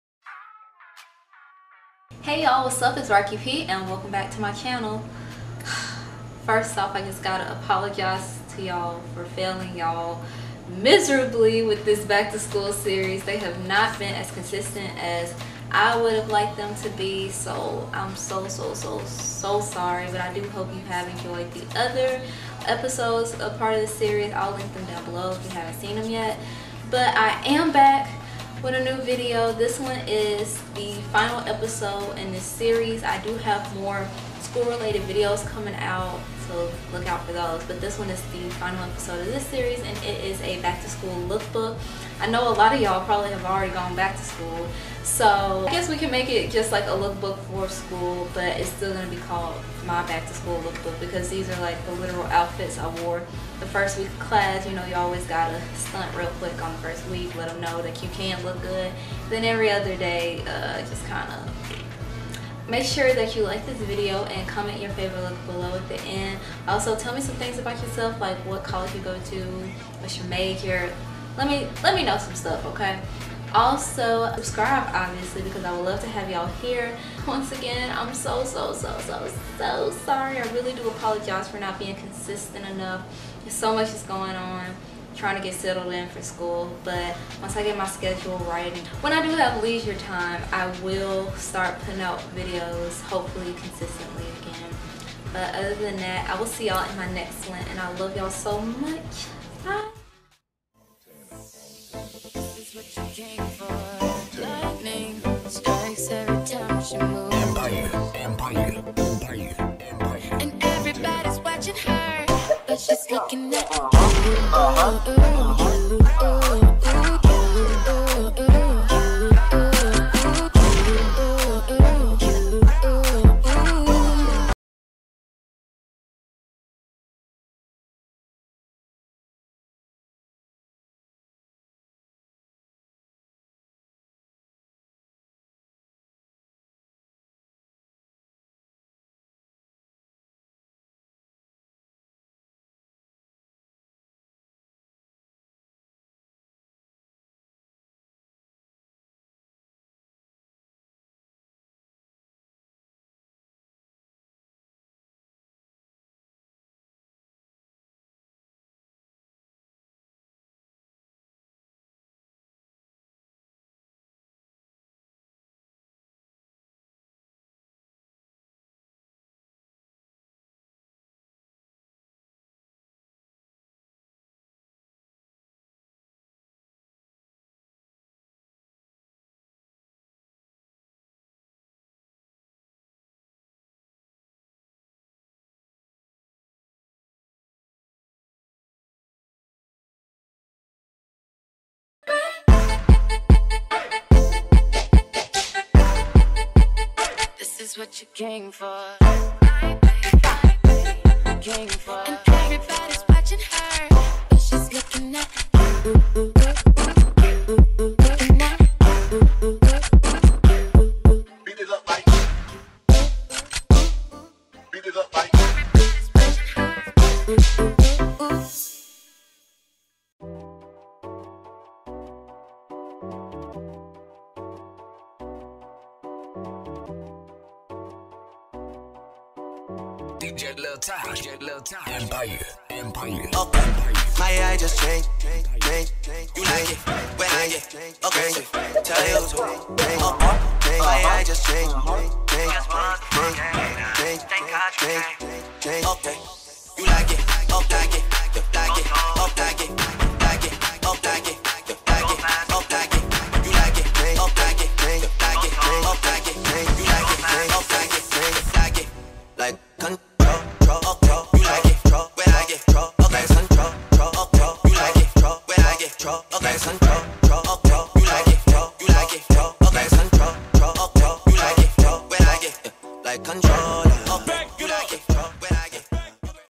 Back To School Lookbook 2016 | Raque P
Guyssss I hope ya'll still love me! I feel like booty for not being consistent with these back to school videos! BUT I hope you guys did enjoy the lookbook which is the last episode of the series!

Outfit Details

Day 1:
 Dress - Forever 21
Shoes- Forever 21
Necklace- Forever 21
Watch- Francessa's
Purse- Dillards (MK)

Day 2
Shirt- H&M
Shorts- Rue 21
Shoes - Forever 21
Kimono- Forever 21

Day 3
Shirt- American Eagle
Jeans- American Eagle
Shoes- Forever 21
Necklace- Forever 21
Wristlet- Fossil

Day 4
Shirt- Rue 21
Pants- American Eagle
Shoes- Payless

Day 5
Shirt- School's Bookstore
Leggings- Papaya
Shoes- Forever 21

v=PC1n8253X5w

Ya'll I honestly can't believe we made it to over 500 subbies by the end of the summer! I'm honestly amazed at all of ya'll support!

raquelpilar

Hair Info: 4b/4c Natural (Never had a perm)
If you've read this far give the video a thumbs up for the helpful info! :D

See you in my next video xo